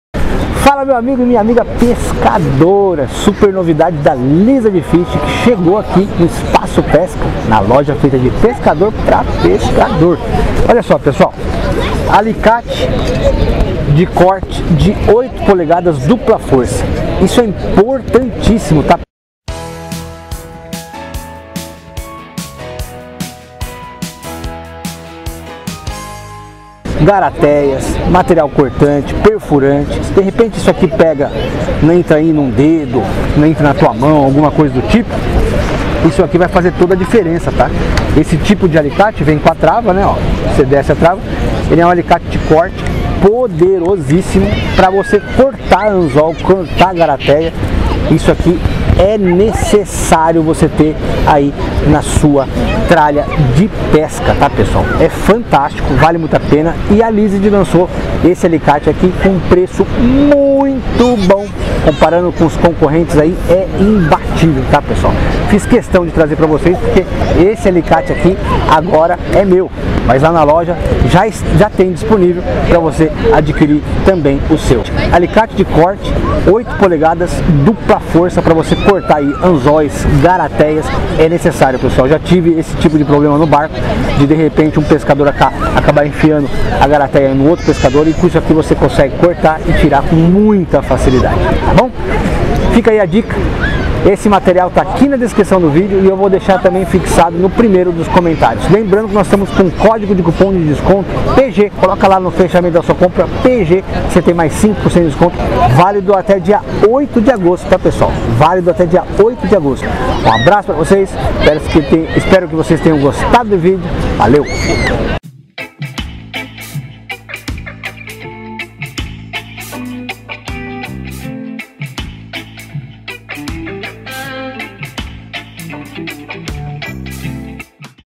Este Equipamento é FUNDAMENTAL na caixa de pesca - Alicate dupla força da Liazard Fishing
Muito boa noite meu amigo Pescador!
Hoje trago para você o Alicate DUPLA FORÇA da Lizard Fishing.
Material de SEGURANÇA de extrema importância.
Alicarte poderoso que corta anzol, garateia etc.
Este Equipamento é FUNDAMENTAL na caixa de pesca!
Confira no Vídeo!
Abraços
P.G
Segue abaixo o link do alicate dupla força da Lizard Fishing, sóclicar para ver mais e poder também adquirir.

▬▬▬▬▬▬ Contatos profissionais  ▬▬▬▬▬▬▬
- Parcerias